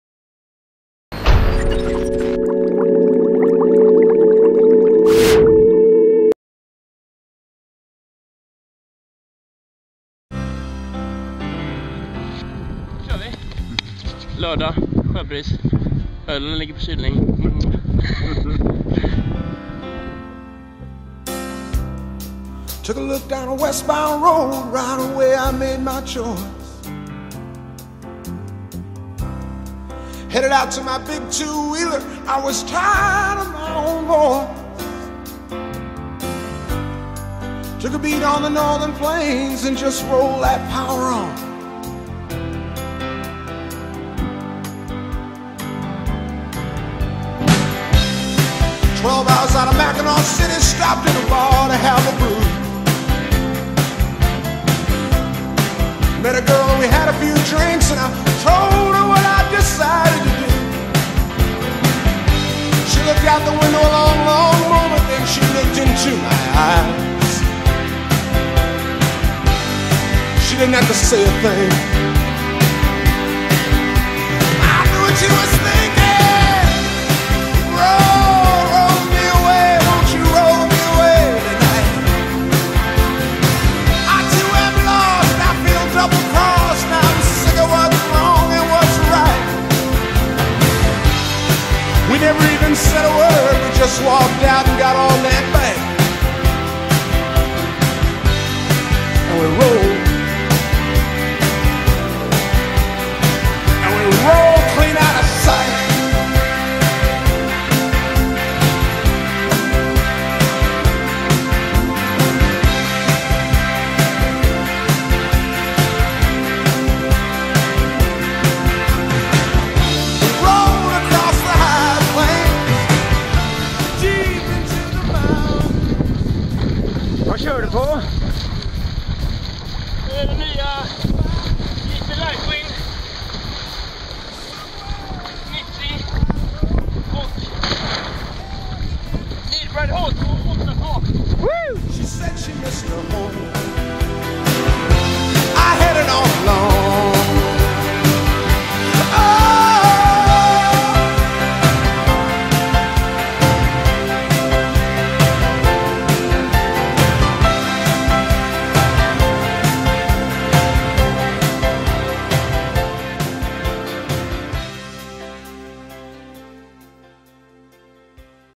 Segling i lättvind med JP Lightwind.
Vindsurfing i apelviken i riktigt lättvind med JP Lightwind och Neil Pryde H2 segel